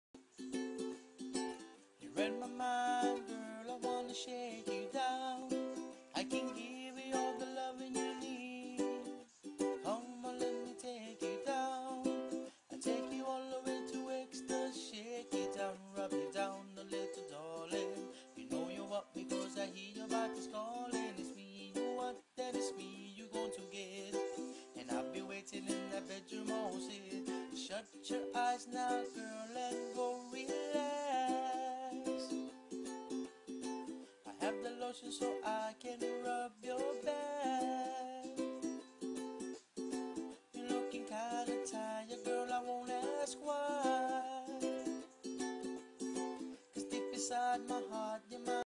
Caught on Video: Rare Wallaby Sighting in Hawaii on Valentine's day
Caught on Camera:Rare Sighting of a Wallaby on the Island of Oahu in Hawaii.